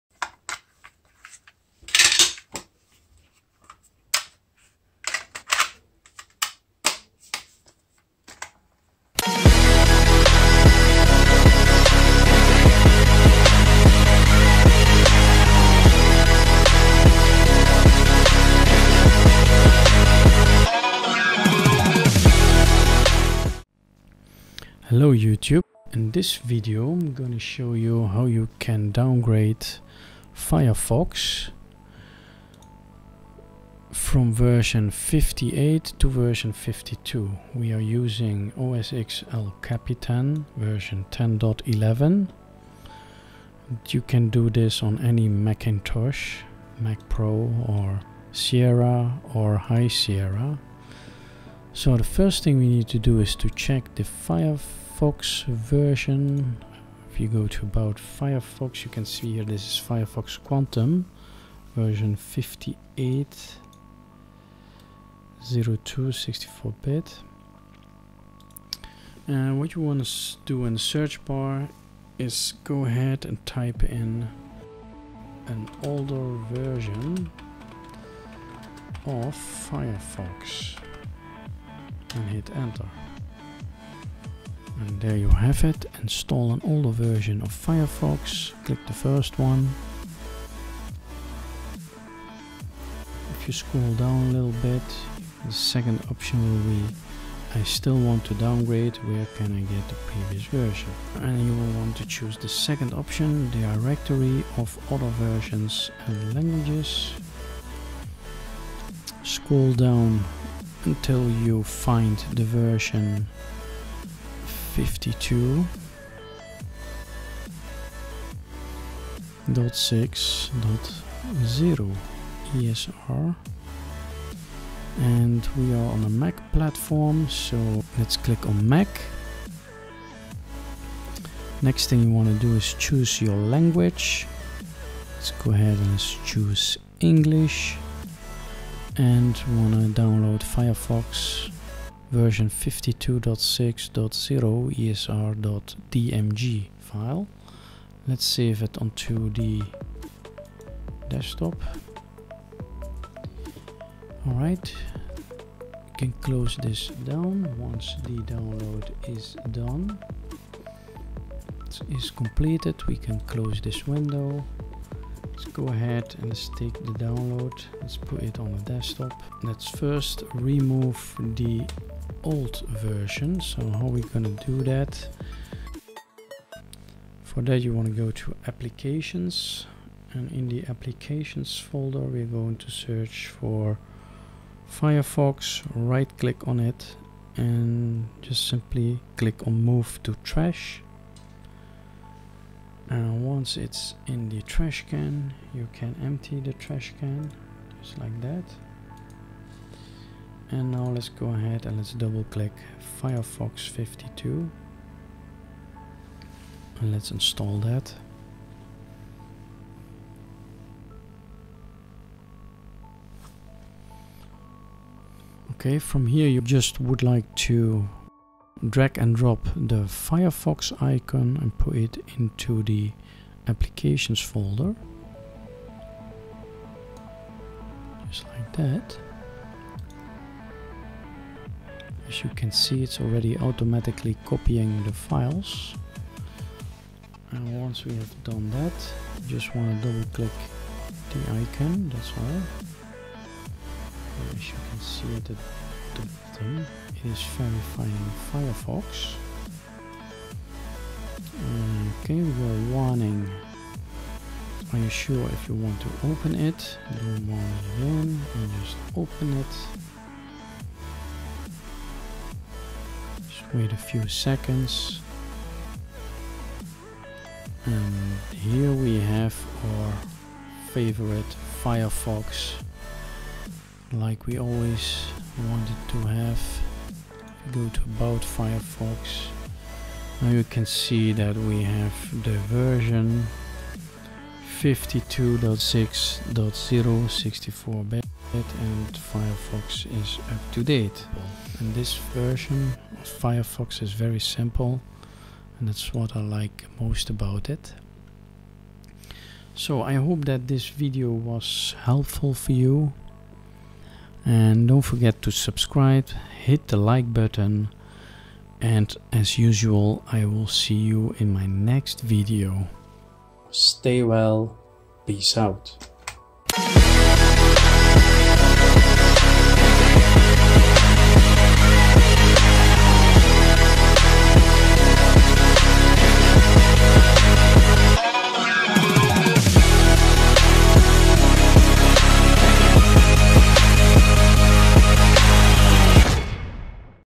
[MAC] How to install Firefox ESR V52 64bit
In this video we are going to downgrade Firefox version 58 to version 52 ESR on Mac OSX El Capitan version 10.11.
If for any reason you want to downgrade Firefox then this video is for you.
The Mac version we use in this video is Mac OSX El Capitan 10.11 but this installation will also work on Mac OSX Sierra and Mac OSX High Sierra.

From the search bar you can also type in the following words:
Install an older version of Firefox

Follow the steps in this video and enjoy!